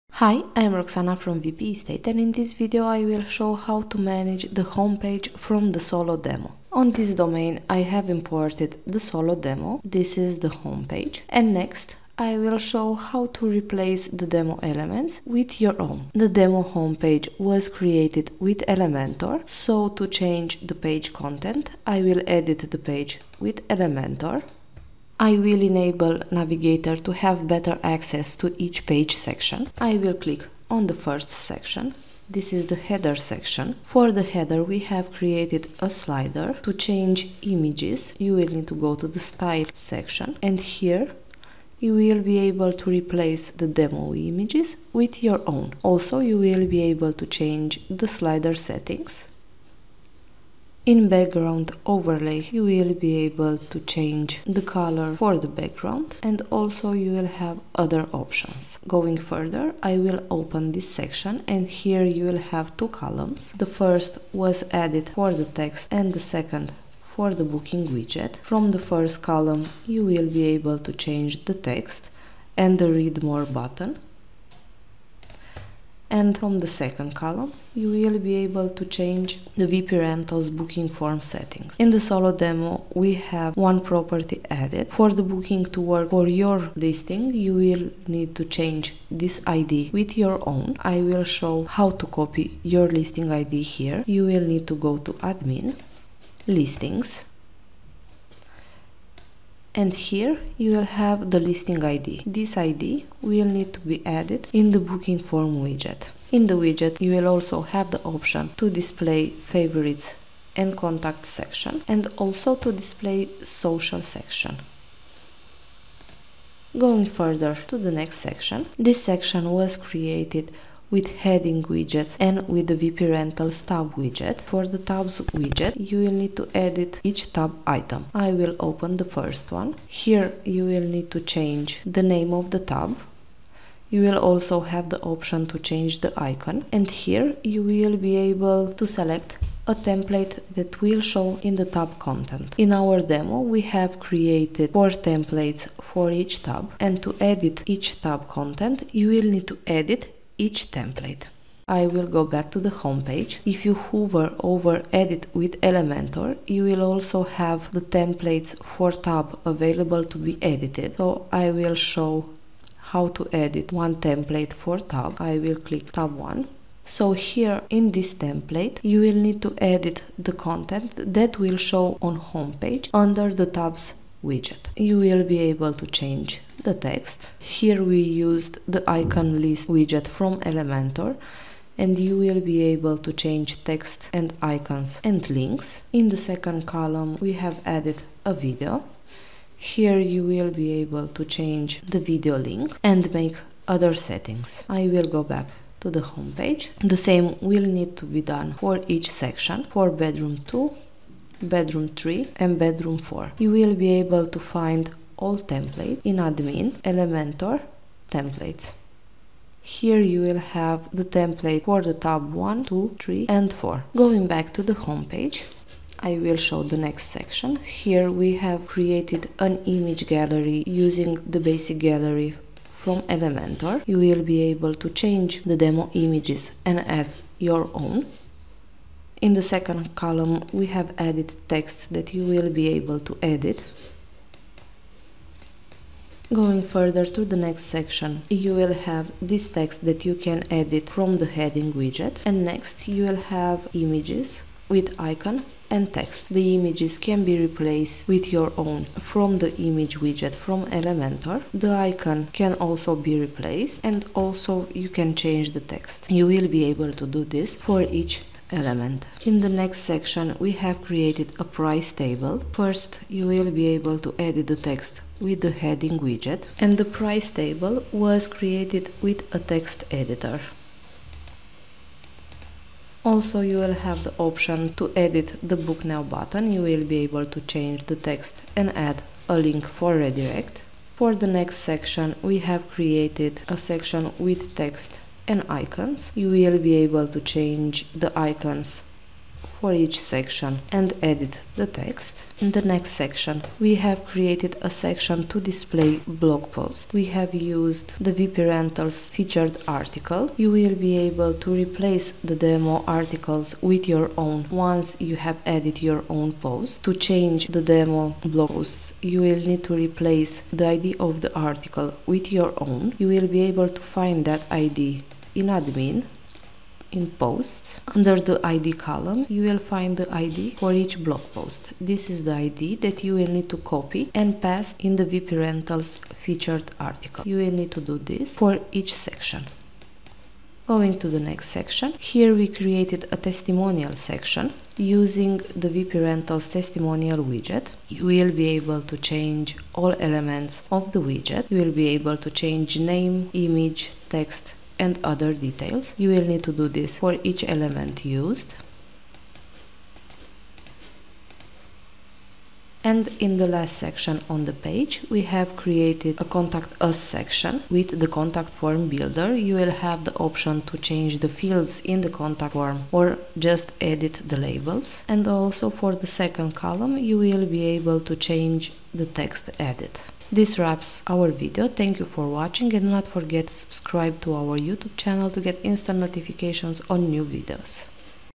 How to edit the homepage from Solo demo -WP Rentals theme
In this video, you find quick guidelines on how to edit the Solo demo homepage.  You will learn how to change the images in the header slider, how to manage the WP Rentals Tabs section, and many other details.